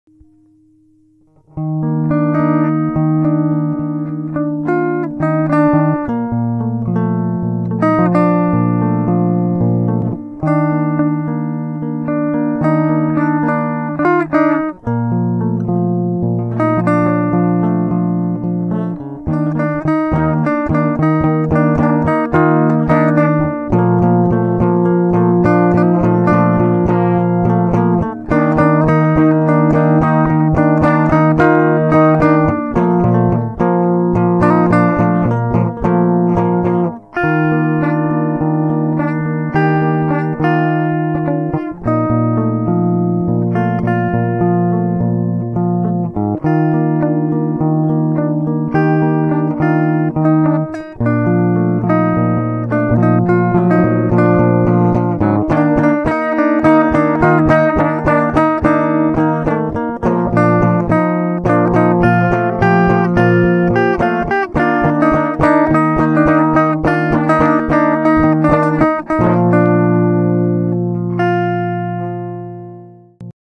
This is What I do When I'm Bored by AlexplaysPad
I got bored and decided to mess with my guitar recorded it.